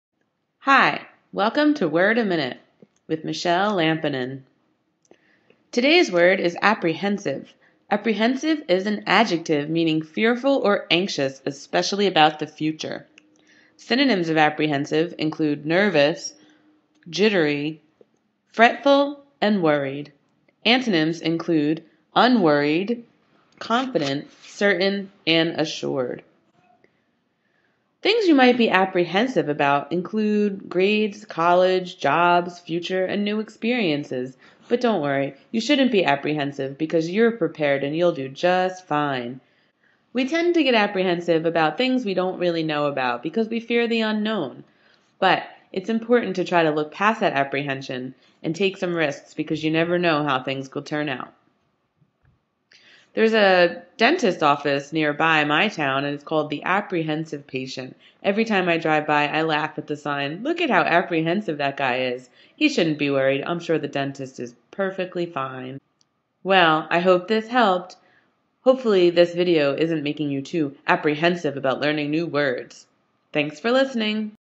Word-a-Minute: apprehensive
apprehensive (adj.): anxious or afraid, especially about the future
synonyms: nervous, jittery, fretful, worried
antonyms: assured, unworried, confident, certain